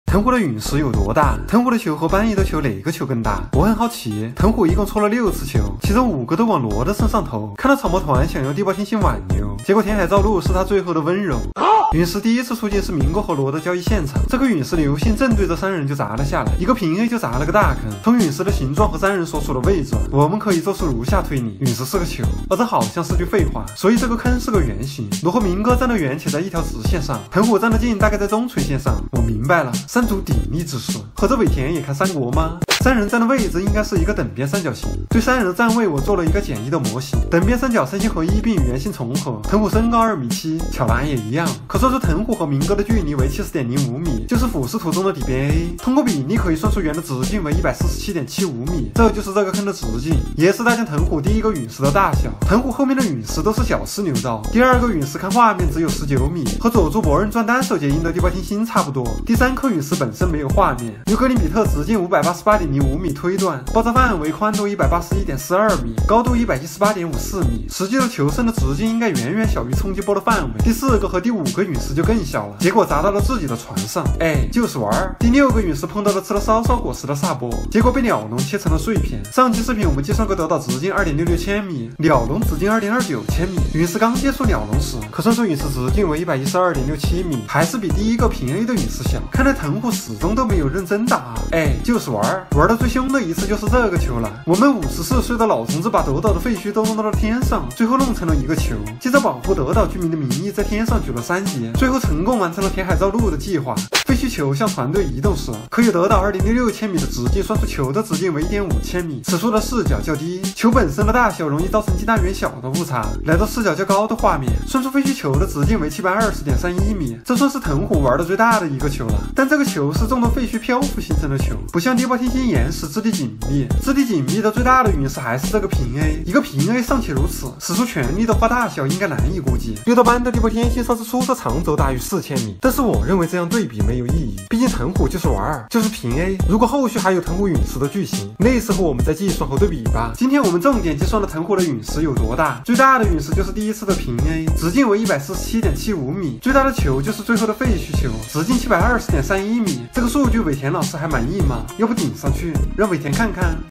我算出了大将藤虎所有的陨石有多大！Youtube第一人【最上川下山】
这是海贼王计算系列的正片
 我算出了大将藤虎所有的陨石有多大！Youtube第一人【最上川下山】娱乐视频非科普，希望你天天开心 请放心，本视频内容符合YouTube社区准则，没有任何违反规定的行为。